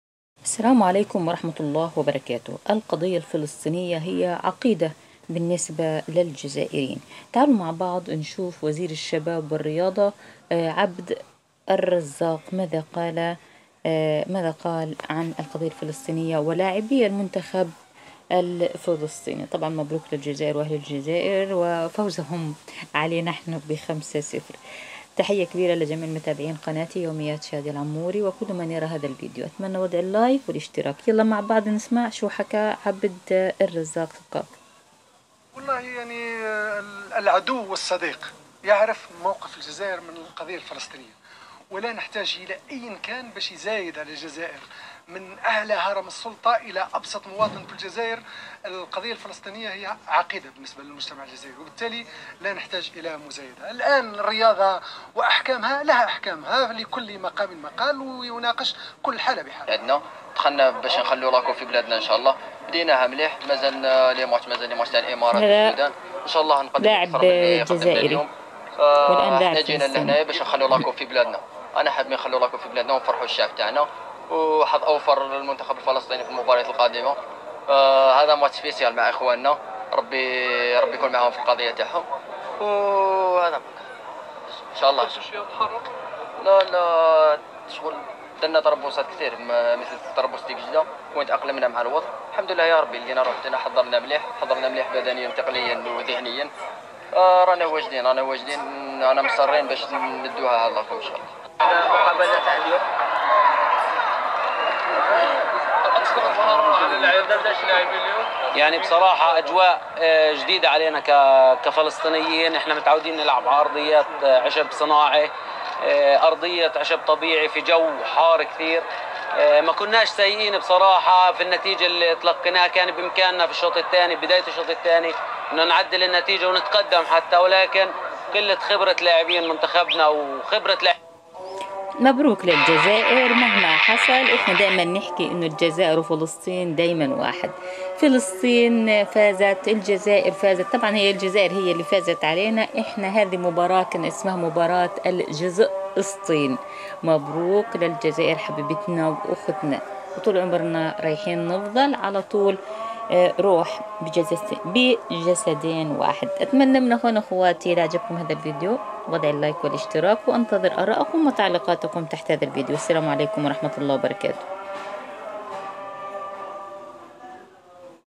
اسمع بالفيديو/وزير الشباب والرياضه الجزائري ماذا يقول عن فلسطين
يوميات_شاديه_العموري هذا ماقاله وزير الشباب والرياضة الجزائري عبد الرزاق سبقاق عن فلسطين
فلسطين,تصريحات عبد الرزاق عن فلسطين,تقرير,الشعب الفلسطيني,وزير الشباب والرياضة التونسي,تاريخ
لاعبي المنخب الفلسطيني
حب الجزايريين لشعب الفلسطيني